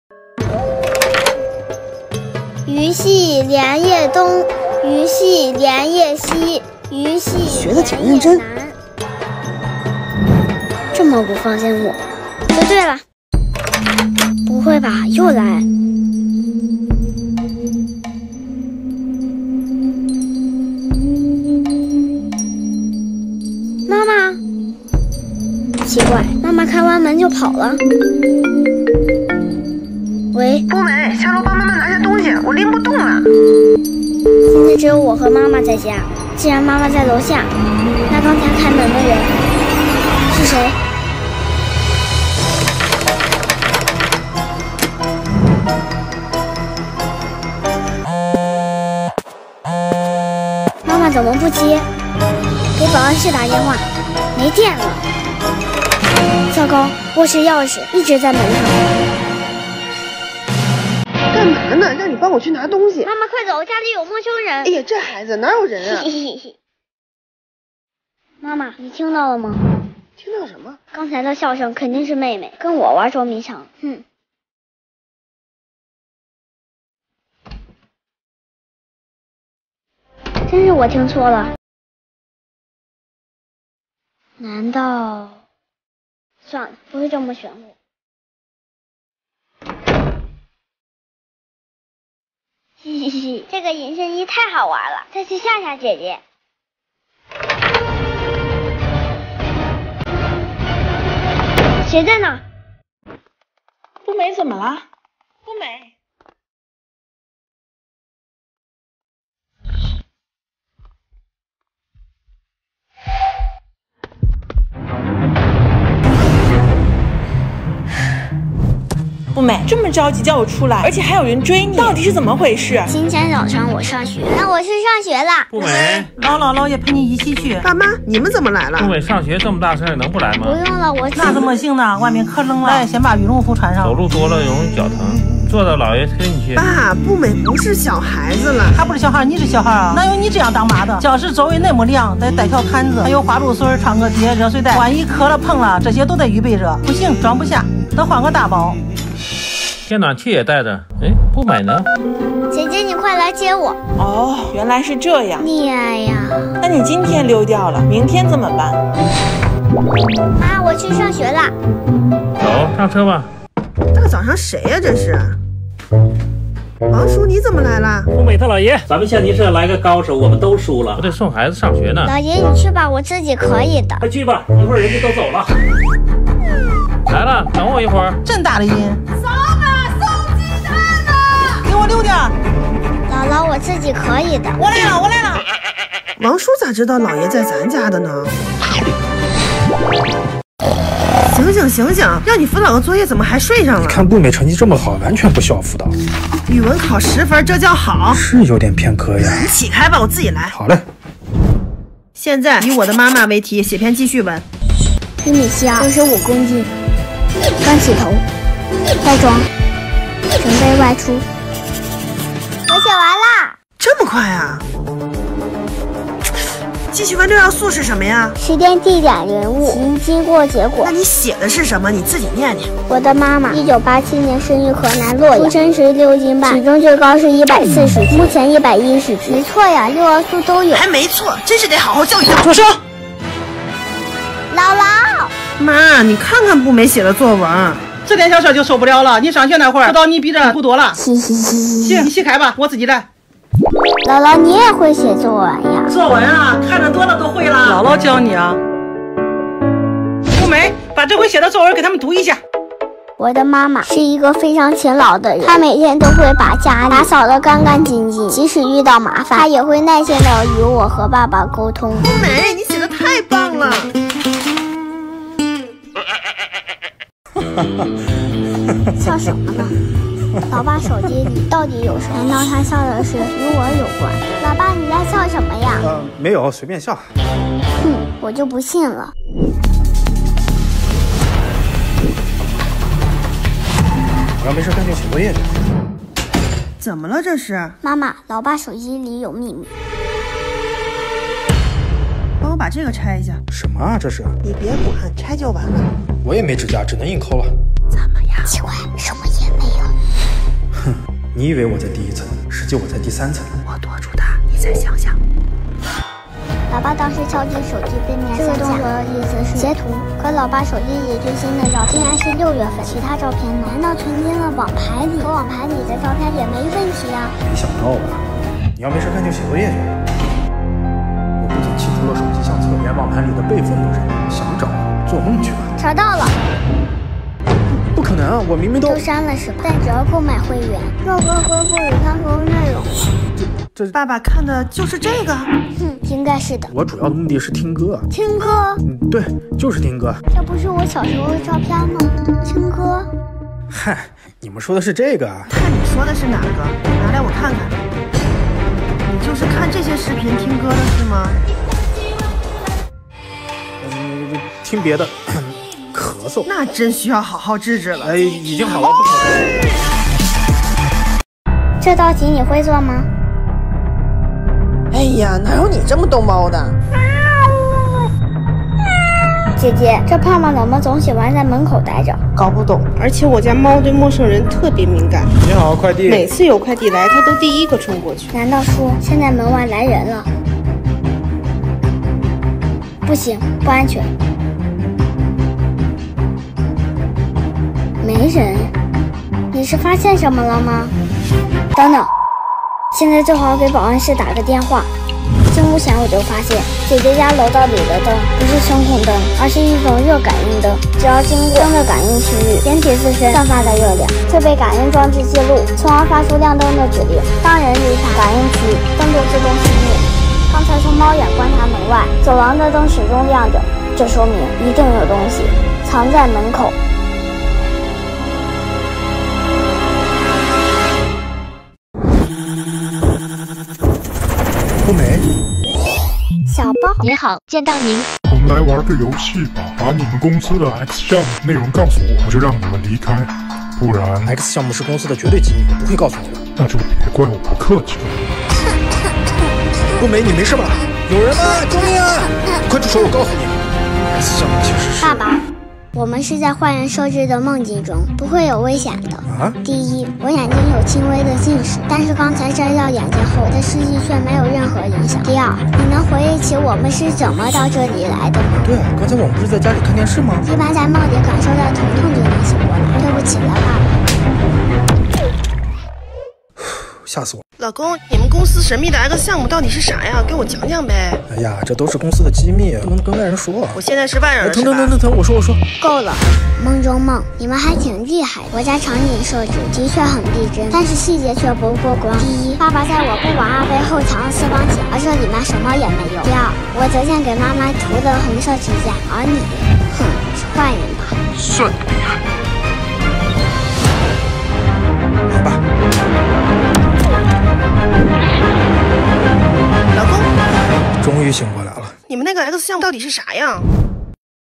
妈妈下楼买菜，步美独自在家，究竟是谁打开了步美的房间门？【名侦探步美】
哈喽，YouTube 观众，大家好...😘
我是步美，帮助所有大朋友小朋友们远离伤害🥰